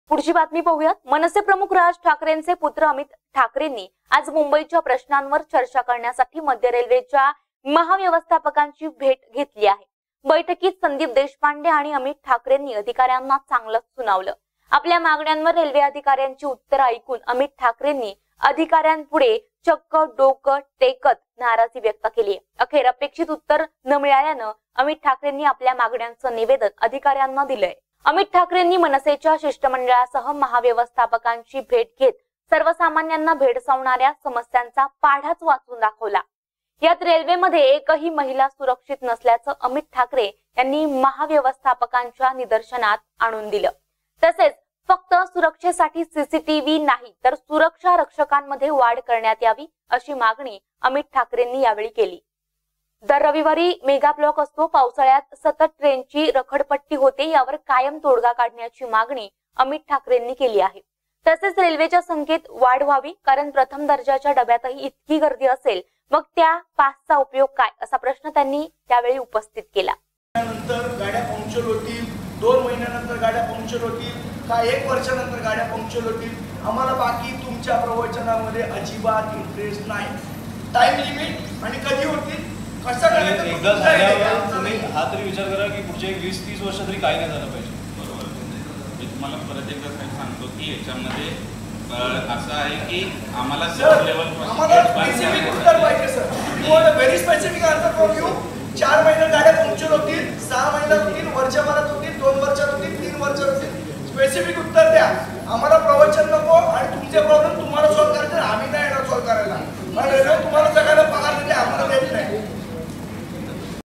अमित ठाकरेंनी अधिकाऱ्यांपुढे डोकं टेकलं
रेल्वे अधिकाऱ्यांकडून अपेक्षित उत्तरं नाही,